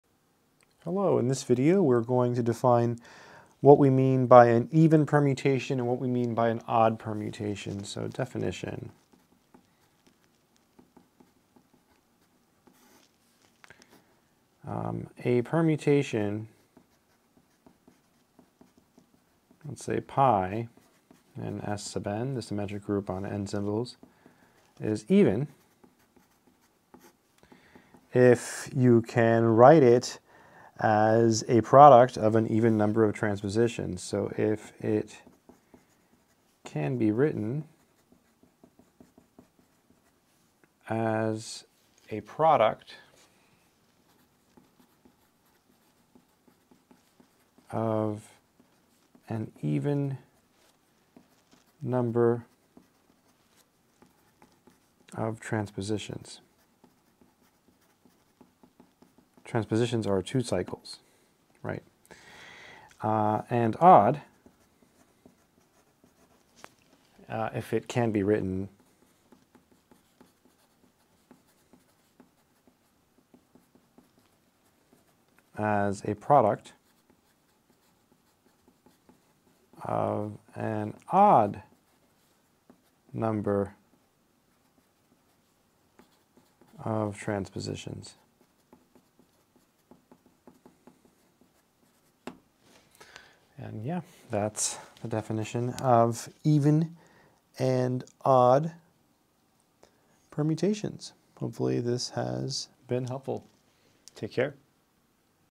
Definition of an Even Permuation and Odd Permutation
We define what it means for a permuation to be even or odd. If we let pi be a permutation in S_n, we say it is even if it can be written as a product of an even number of transpositions, and it is odd if we can write it as a product of an odd number of transpositions.